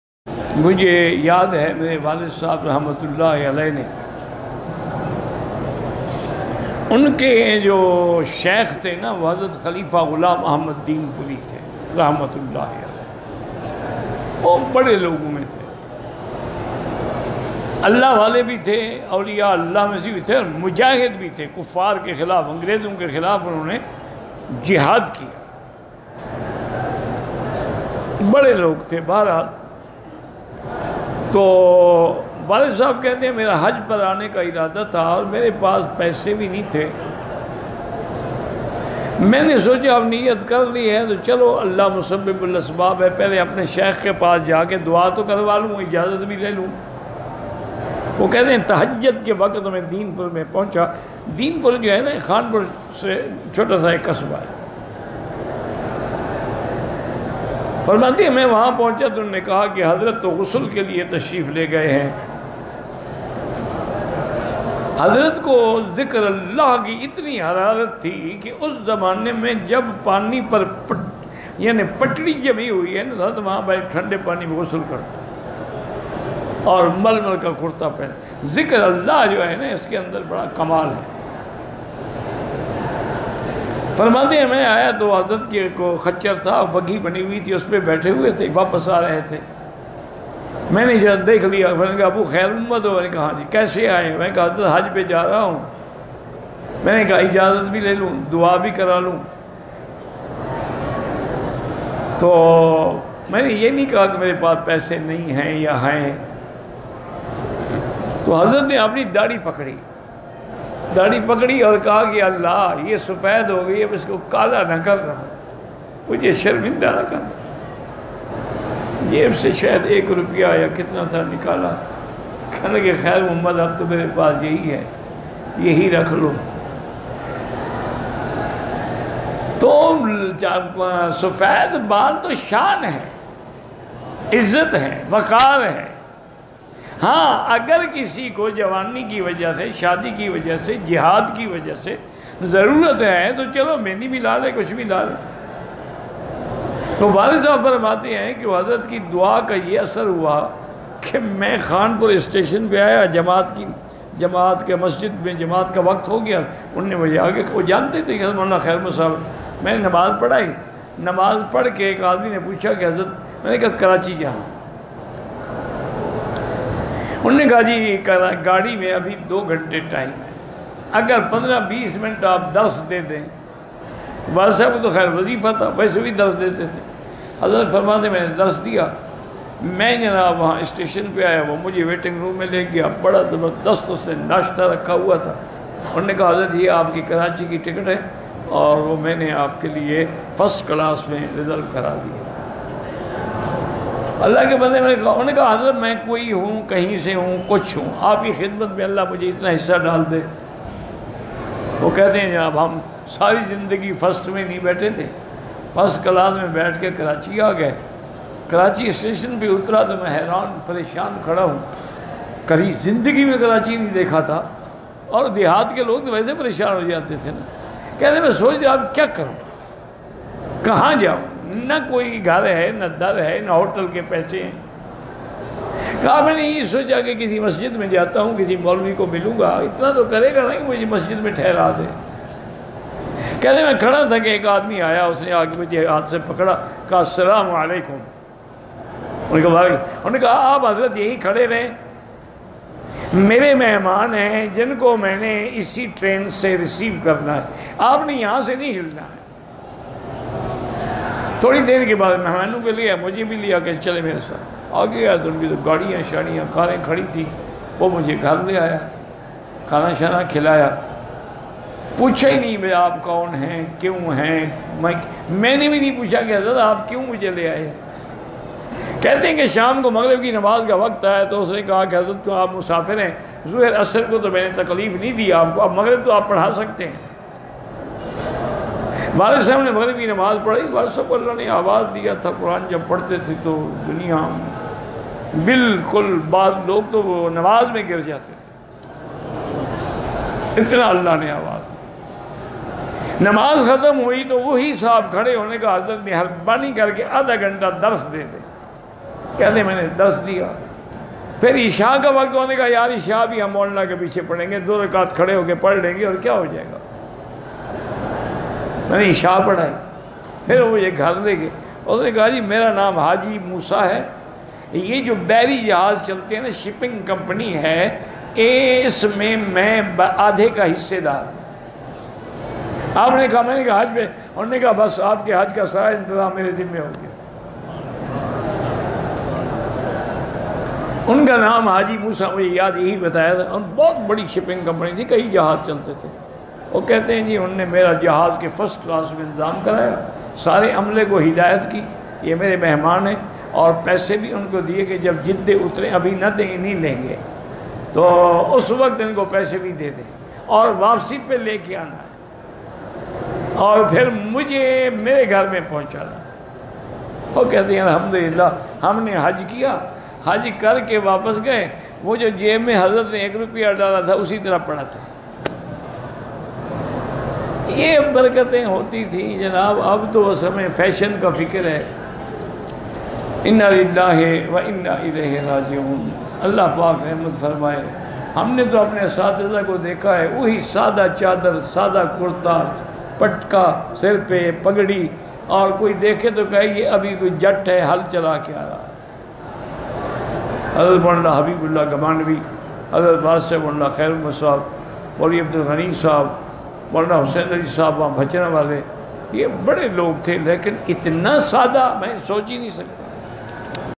Maulana Makki Al Hijazi Ke Walid Sahab Ka Hajj
Maulana Makki Al Hijazi Ke Walid Sahab Ka Hajj Ka Waqia Bayan Kya As Video mein Hazrat Ne